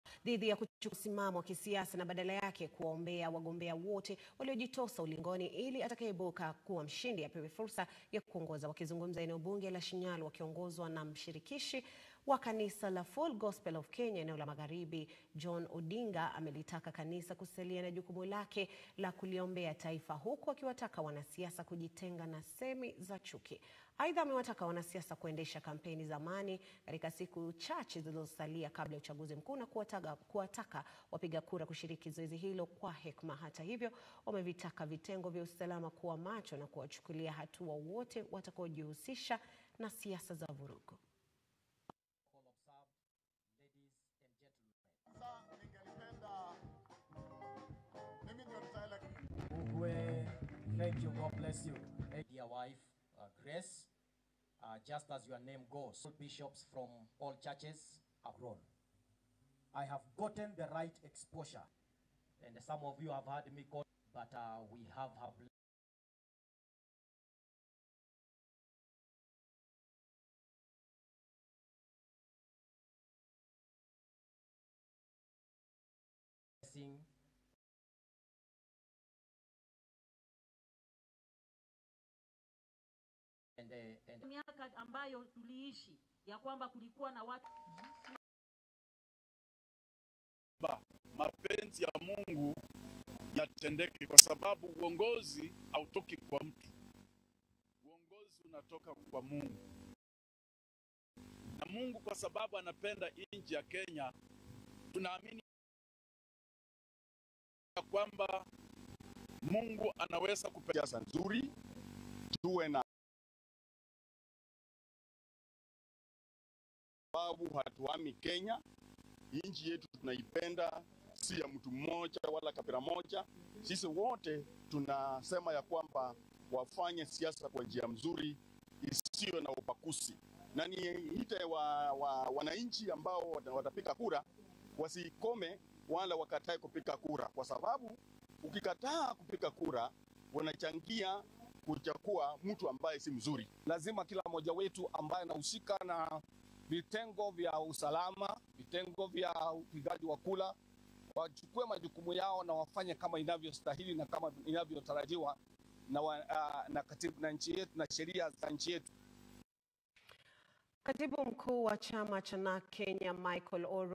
Wito wa amani Kakamega
Viongozi wa dini kaunti ya kakamega wameraiwa dhidi ya kuchukua misimamo ya kisiasa na badala yake kuwaombea wagombea wote waliojitosa ulingoni ili atakayeibuka na ushindi apewe fursa ya kuongoza.